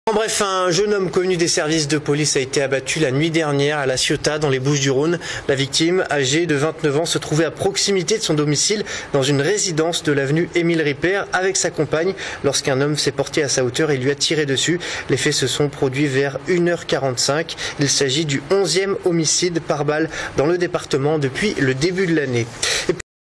Règlement de compte à la Ciotat
Sites webs/blogs, copiez cette vidéo avec le bouton "Intégrer" !
Un jeune homme connu des services de police a été abattu à la Ciotat. Pour exploitation TV, droits réservés, nous contacter